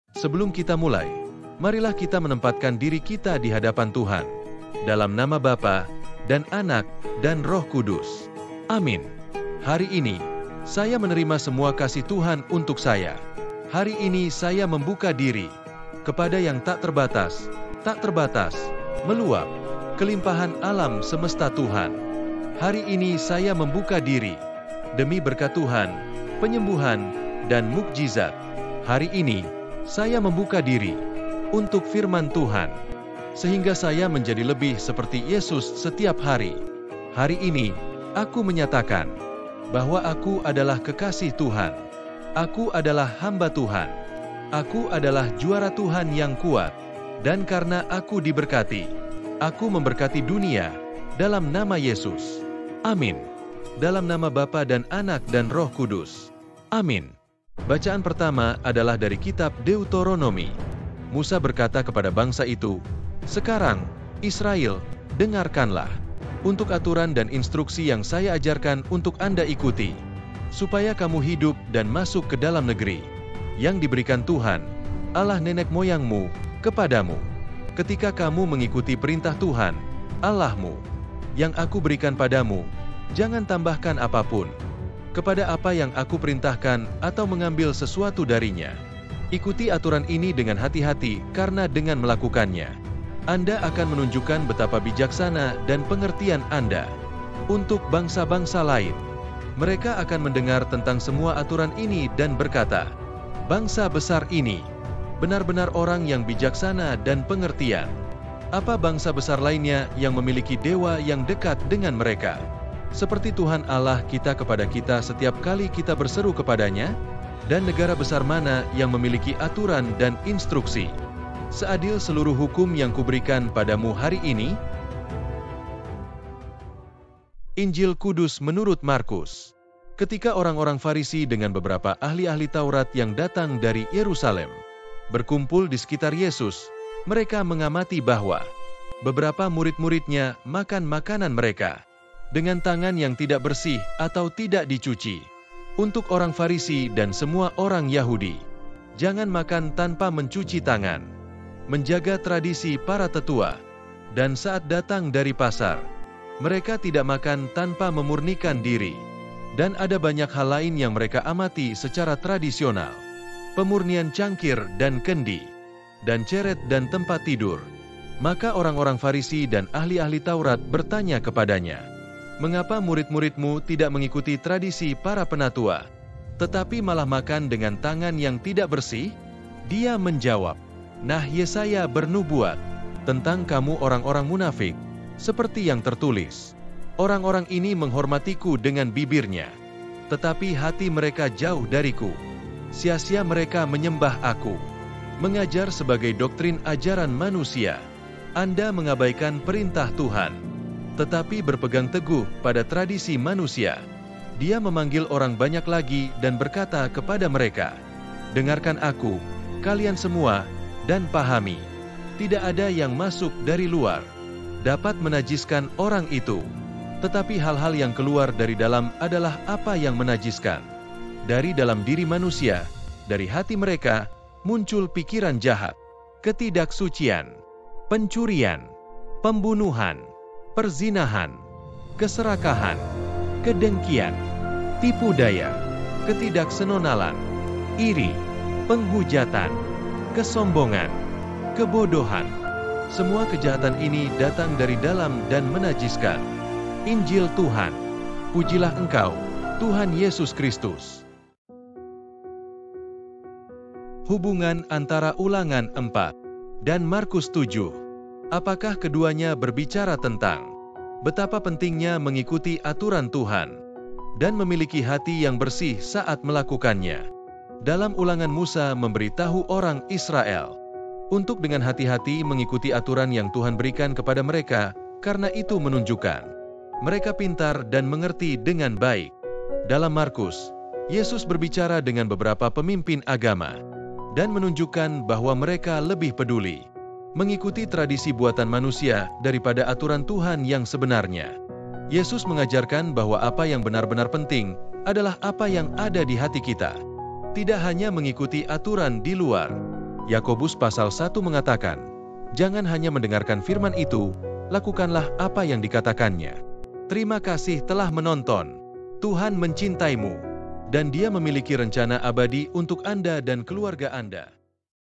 [INDONESIAN] ✝️ ULANGAN 4 DAN MARKUS 7 PELAJARAN SEKOLAH MINGGU KATOLIK 1 September 2024
Minggu Biasa ke-22 Pelajaran Sekolah Minggu Katolik: Hubungan antara Ulangan 4:1-2, 6-8 dan Markus 7:1-8, 14-15, 21-23 adalah bahwa keduanya berbicara tentang betapa pentingnya mengikuti aturan Tuhan dan memiliki hati yang bersih saat melakukannya. Dalam Ulangan, Musa memberi tahu orang Israel untuk dengan hati-hati mengikuti aturan yang diberikan Tuhan kepada mereka karena itu menunjukkan bahwa mereka bijaksana dan mengerti dengan baik. Dalam Markus, Yesus berbicara kepada beberapa pemimpin agama dan menunjukkan bahwa mereka lebih peduli untuk mengikuti tradisi buatan manusia daripada aturan Tuhan yang sebenarnya. Yesus mengajarkan bahwa yang benar-benar penting adalah apa yang ada di hati kita, bukan hanya mengikuti aturan di luar.

Ayat Alkitab Anchor: Yakobus 1:22 mengatakan: "Jangan hanya mendengarkan firman; lakukanlah apa yang dikatakannya."

00:00 Doa pengantar: Novena untuk Kasih Tuhan oleh Bo Sanchez
00:52 Bacaan Pertama dari Ulangan 4:1-2, 6-8
01:45 Bacaan Injil dari Markus 7:1-8, 14-15, 21-23
03:29 Renungan Injil Minggu, 1 September 2024